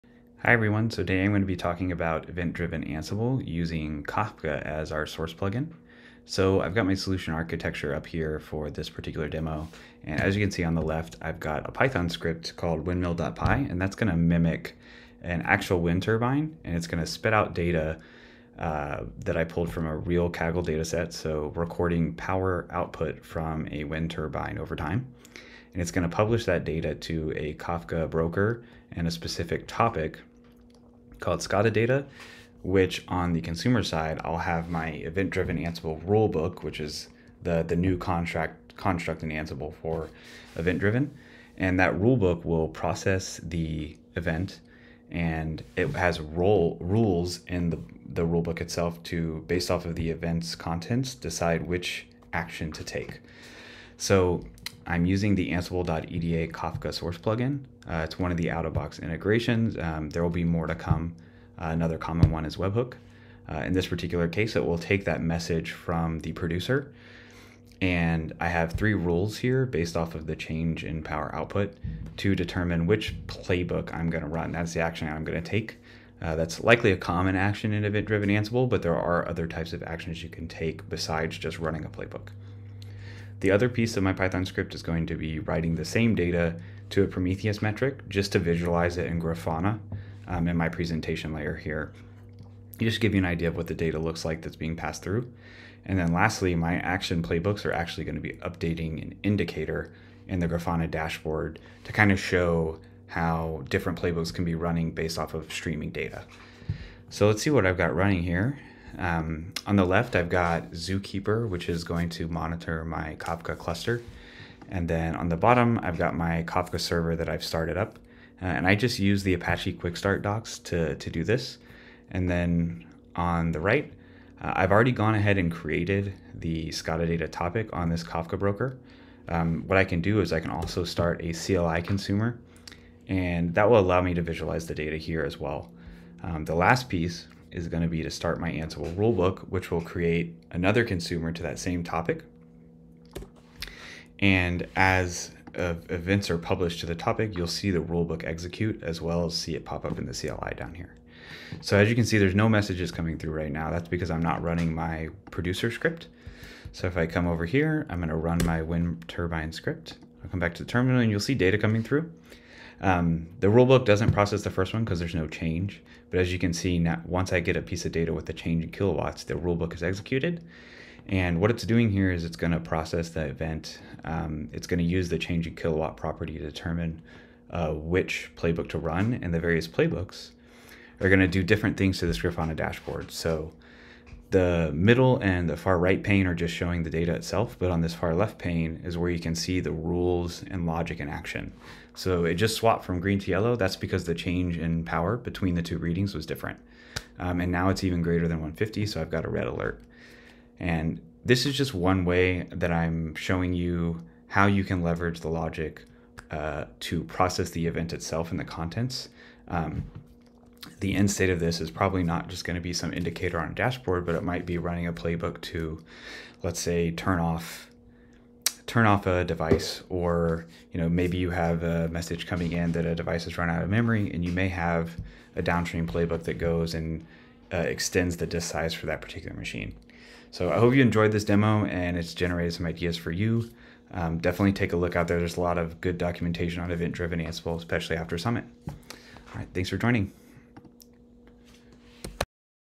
Event-Driven Ansible: Kafka
Demo of Event-Driven Ansible (EDA) using Kafka streams with a SCADA producer representing a real-world wind turbine. The data is simultaneously published to a topic and sent to prometheus for visualization purposes. A Grafana dashboard is used to show both the data stream from my "wind turbine" and to demonstrate how a rulebook in EDA can use logic to execute different actions. In this case, the action taken is determined based on the change in kW observed and will update an indicator in the Grafana dashboard.